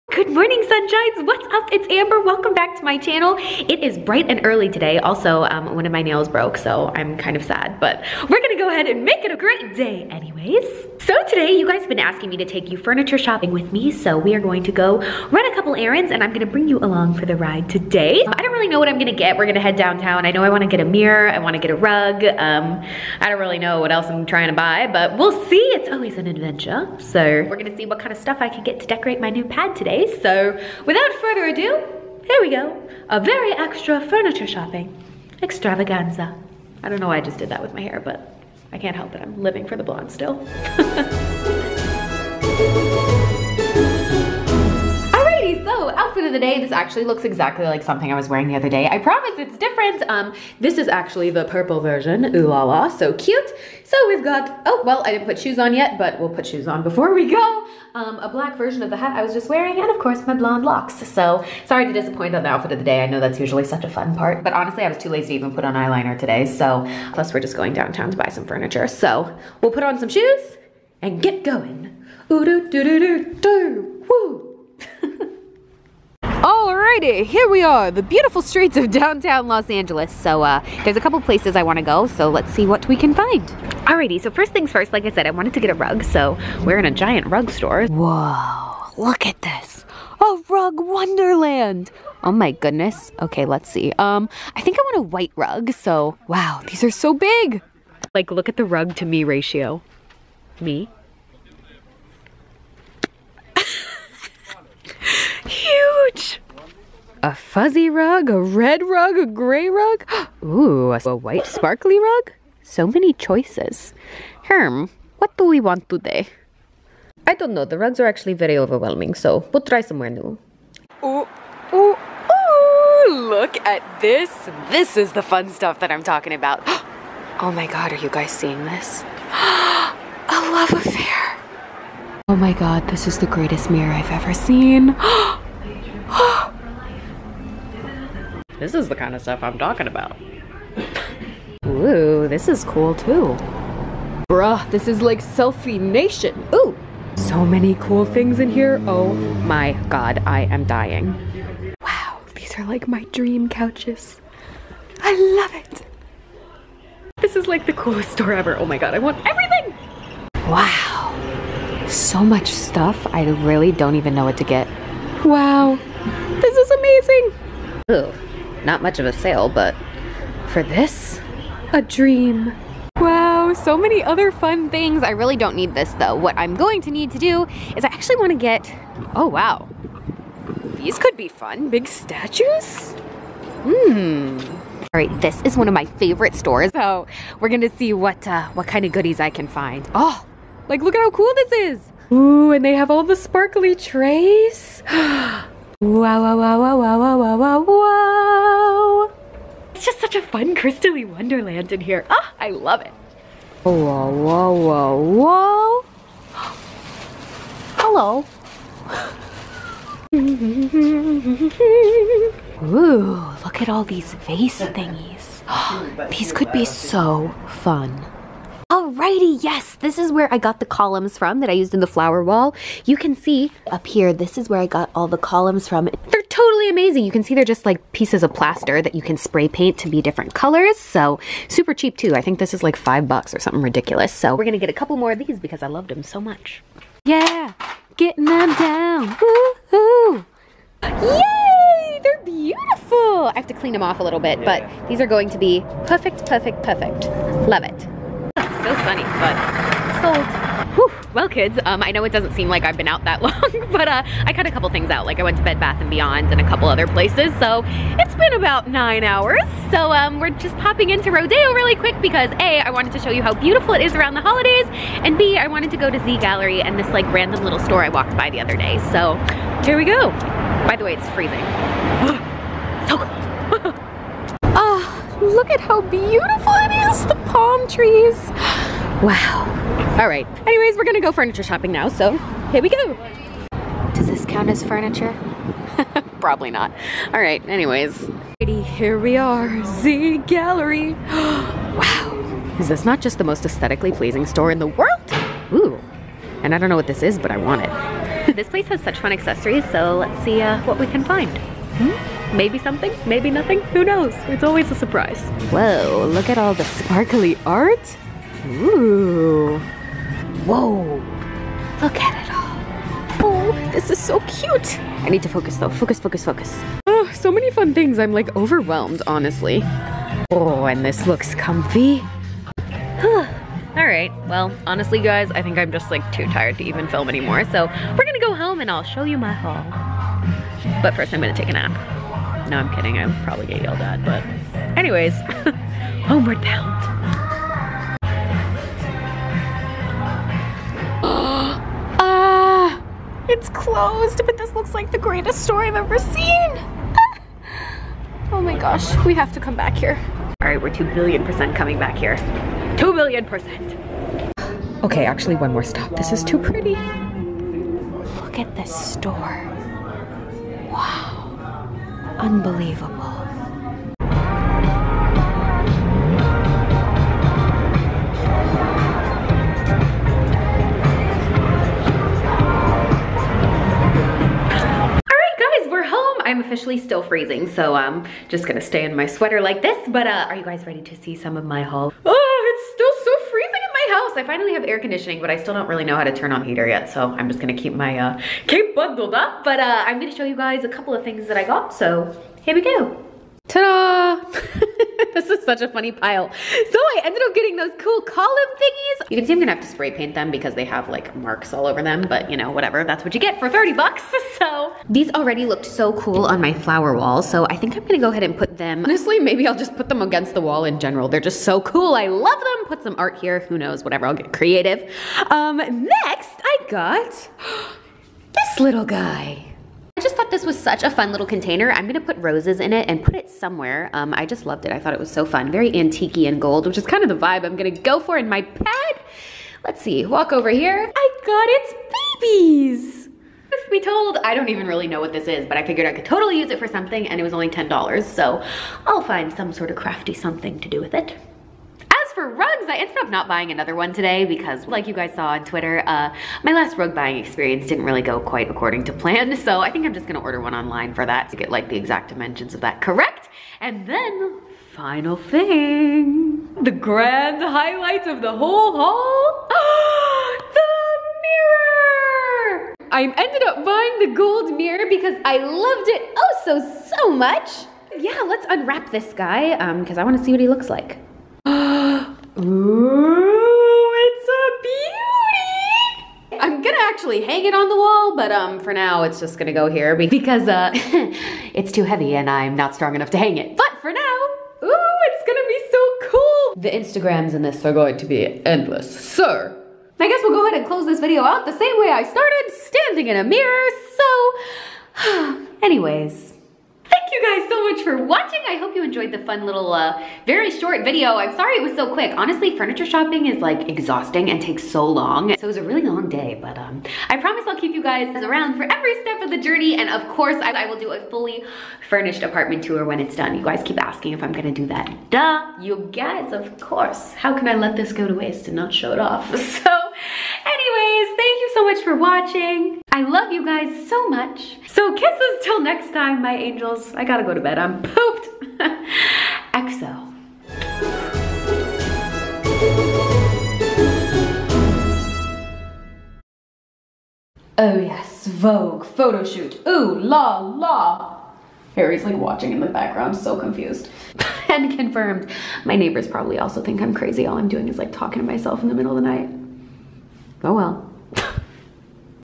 FURNITURE SHOPPING FOR MY NEW APARTMENT!
MERRY CHRISTMAS SWEET ANGEL PIES!!!

Today we are going on a furniture shopping adventure!!! Because I bought a house!!

lol jk but you know I just moved into my new dream apartment, and it needs some home decor! (mostly because I want to do a furnished apartment tour soon and I haven't even bought a couch yet lol) So today we're going to all my favorite cheap (and fancy!) shops searching for extra fabulous decorations!

 ENJOY THE CRAZY-MOVING-SHOPPING ADVENTURE VLOG!!! :P

1,244,000 kisses!
Xx
Amber